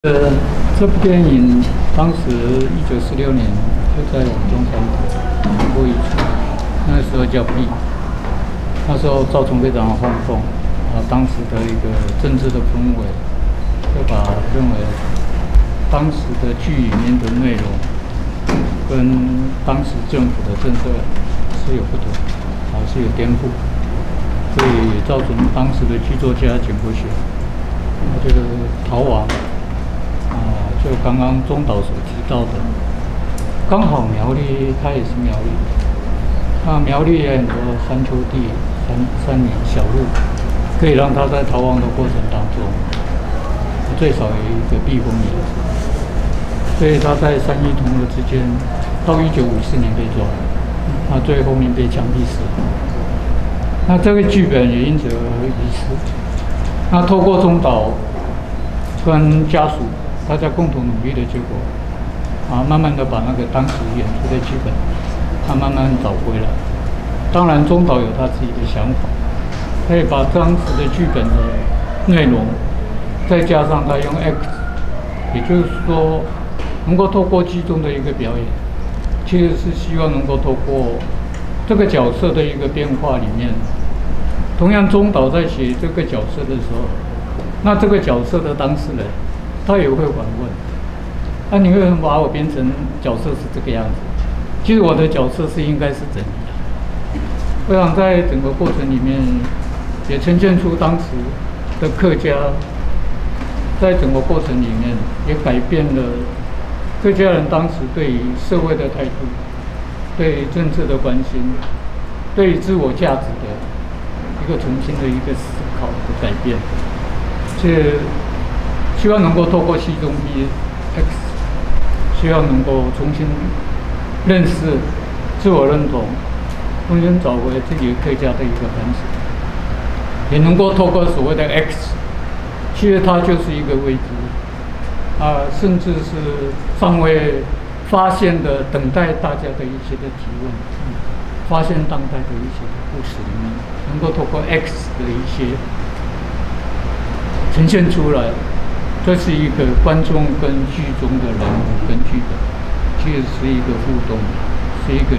鍾孔炤接受媒體聯訪(戲中壁X記者會)2021年8月30日
「壁」這齣戲在228 前夕遭到禁演的厄運。一年後的「戲中壁X」則是全新的版本，讓歷史記憶以當下的情境與面貌，透過劇中腳色跳出劇情，而與劇作家產生持續的對話。